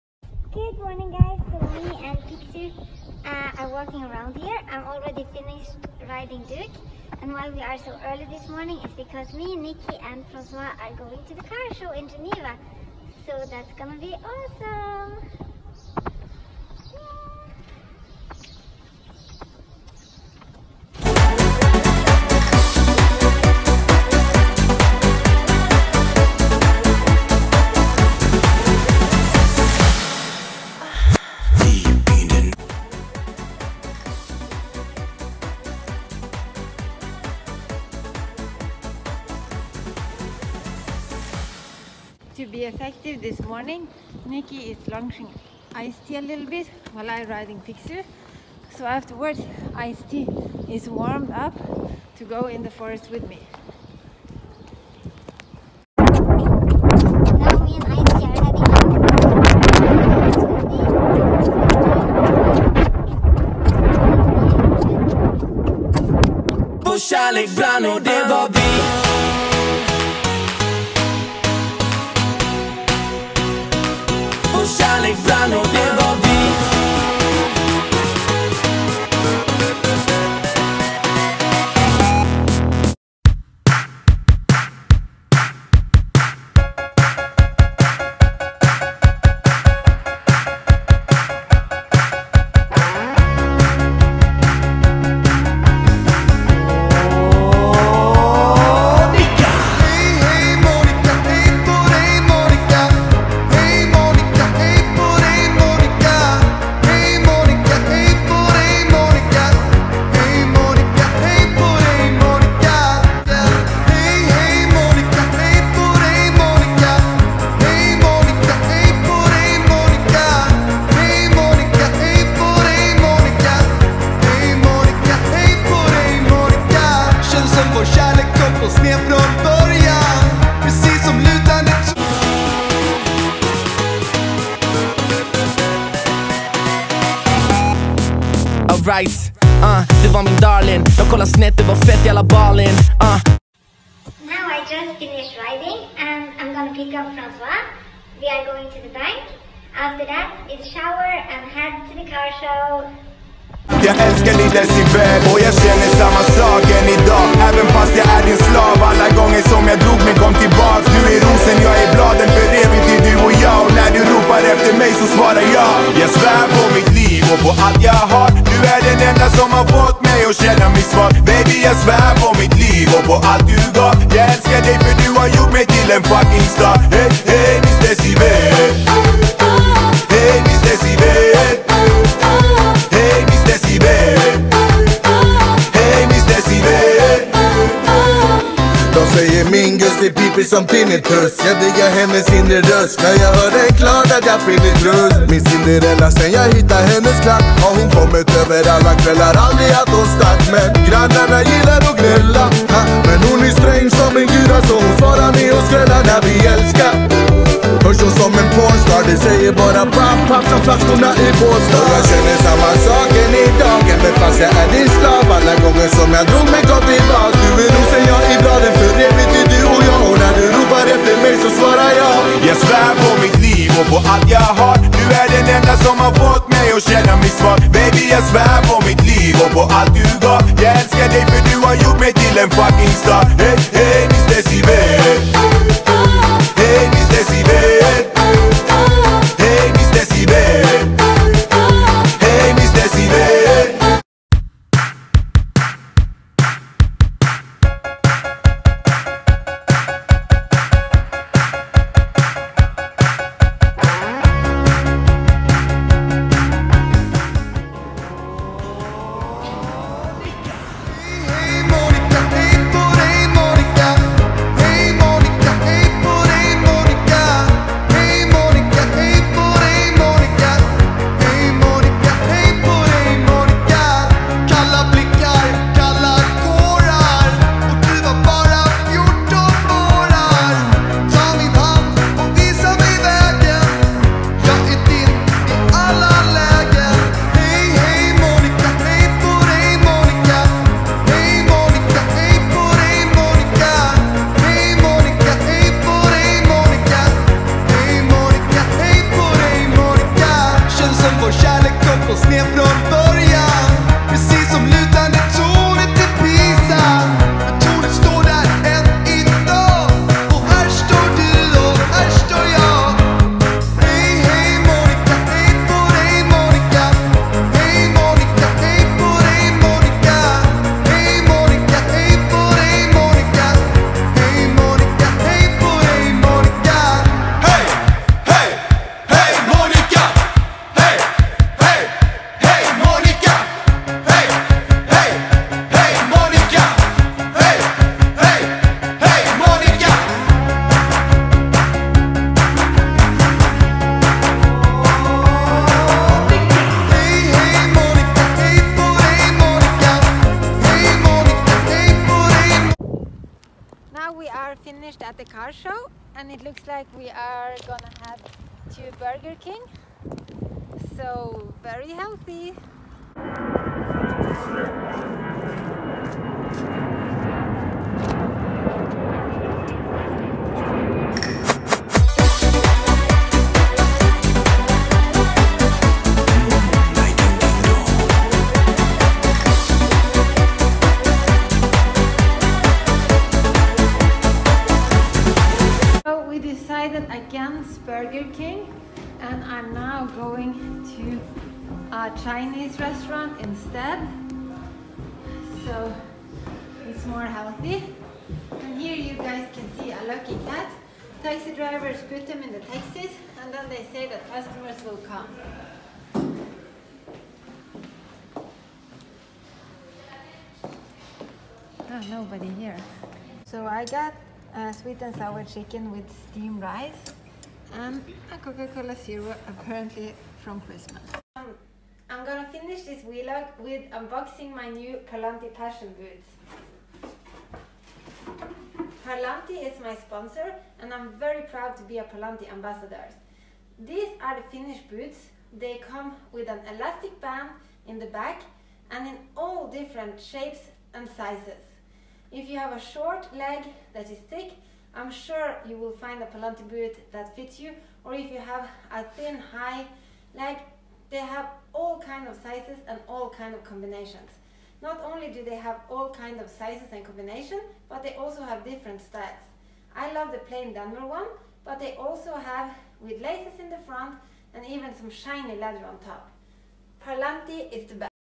VLOG, riding, Salon in Geneva and unboxing Parlanti boots
Tag a long on a day with me, ❤️Francois, Niki and the horses!